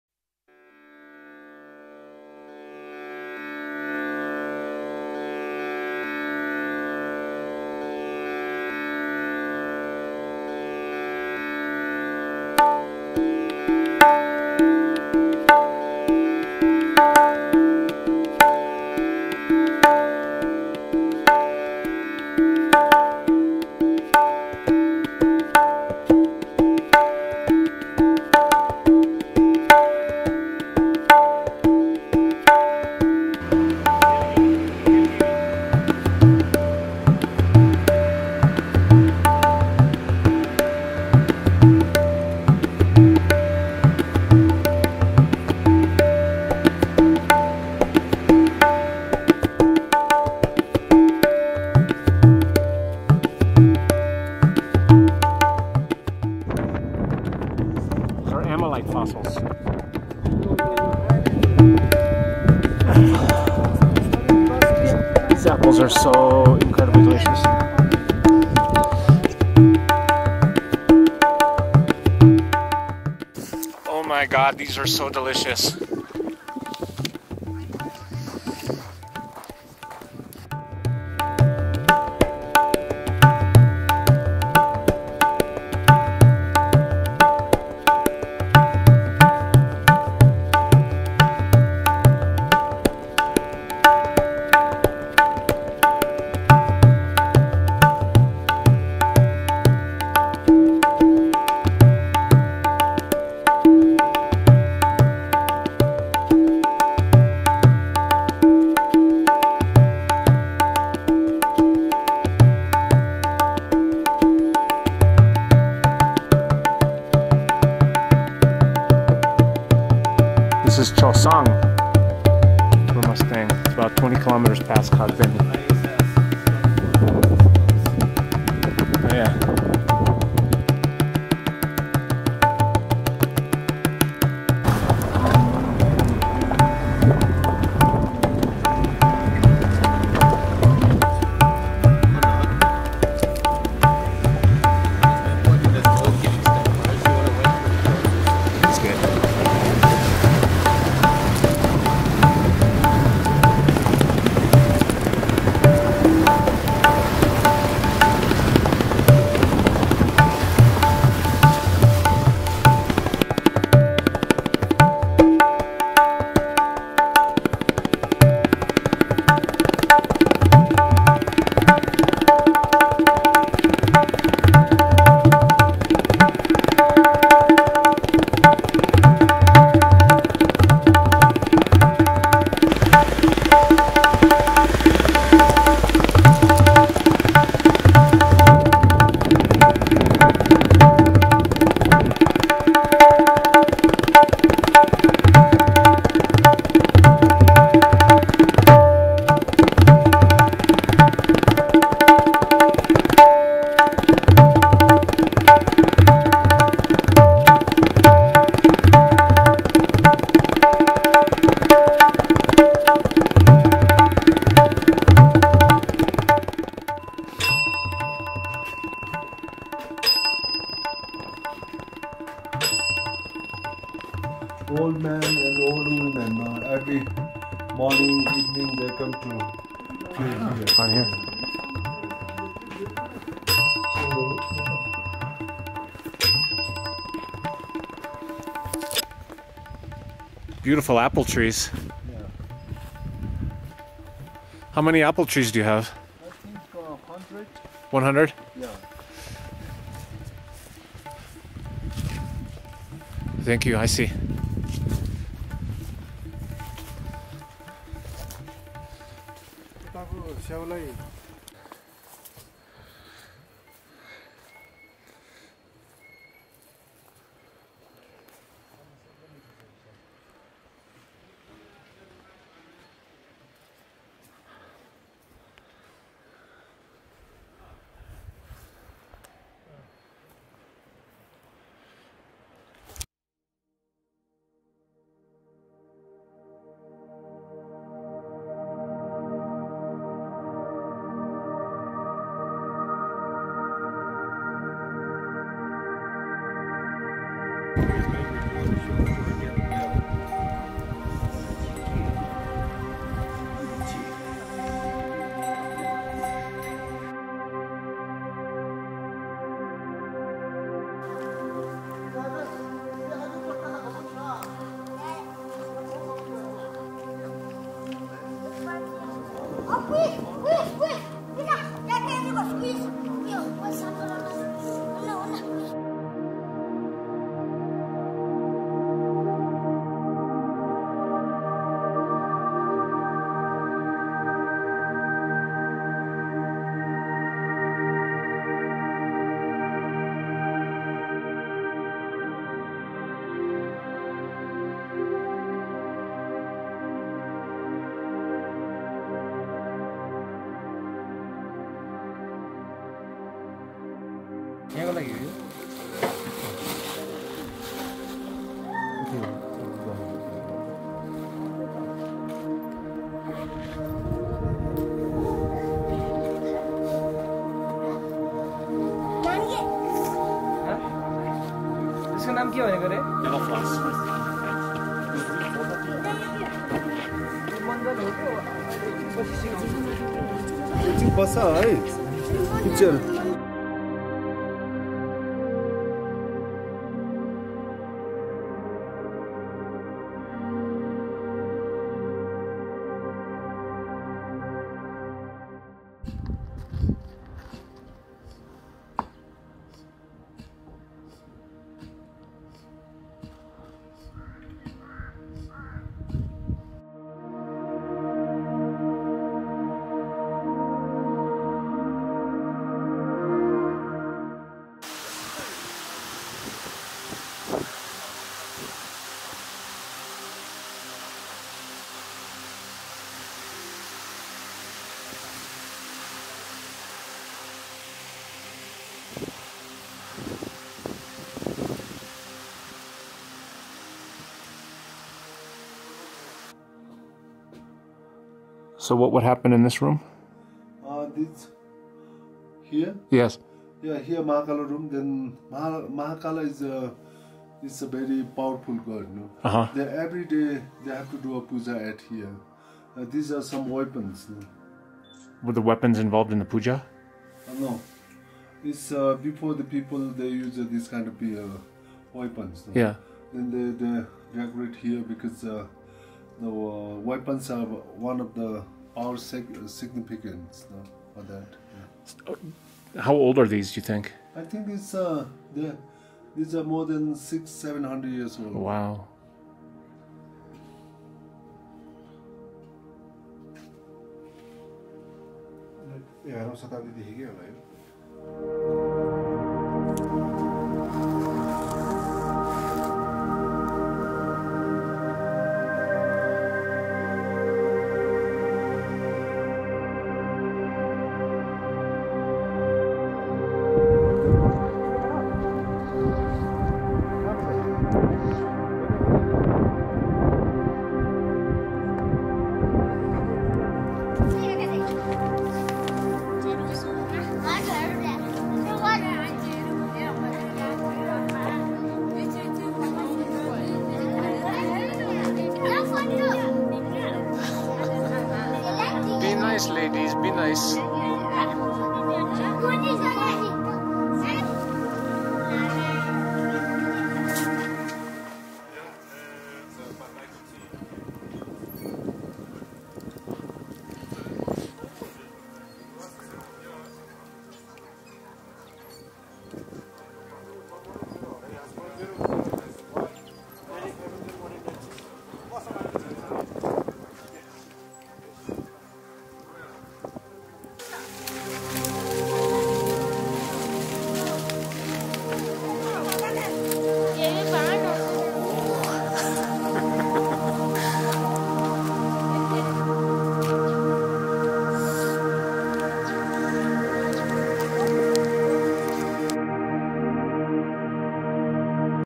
Upper Mustang Nepal - Kingdom of Lo
(Reupload) Trek into the Upper Mustang Restricted Area of Nepal, partly on foot, partly on 4x4.